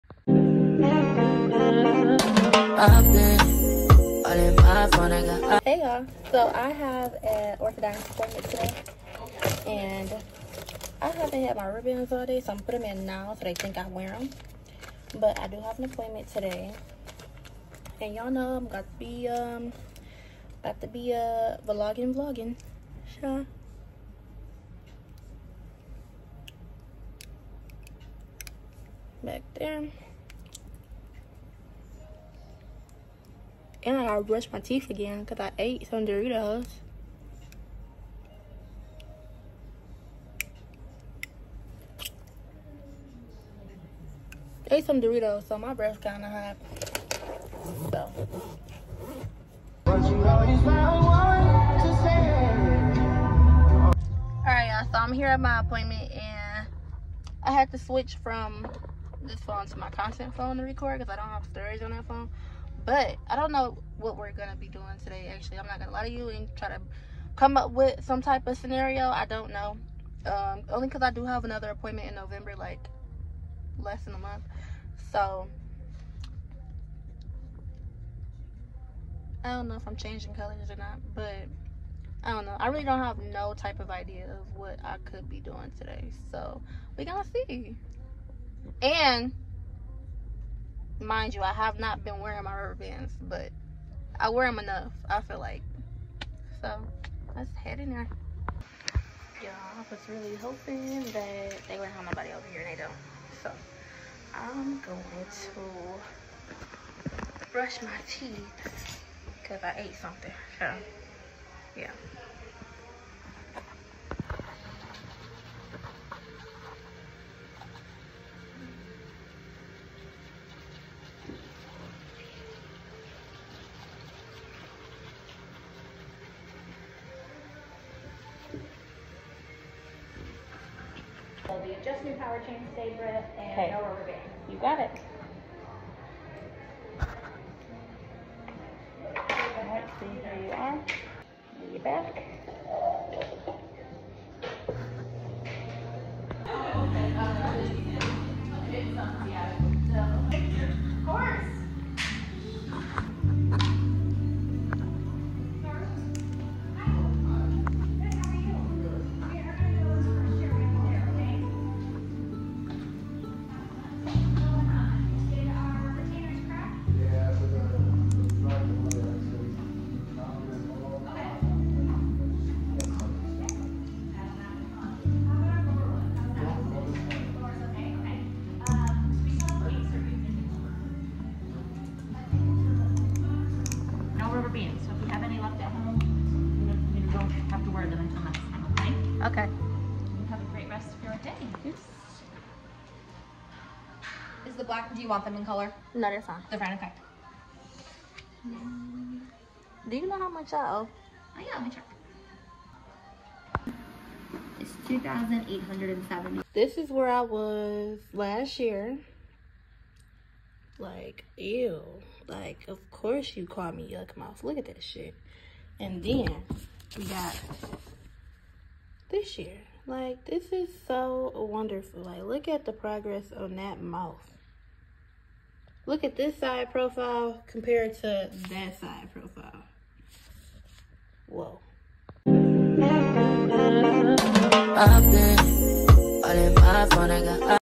9 MONTHS LEFT WITH BRACES! No more PAINFUL elastics
I got braces!! I've been waiting patiently to get my grill together. I'm so happy to be starting my braces journey and seeing results.
I'll be doing some updates to show my progress.

 SOCIAL MEDIAS!

A Divas Everything Boutique “LITTAMONEA” or “LITTA”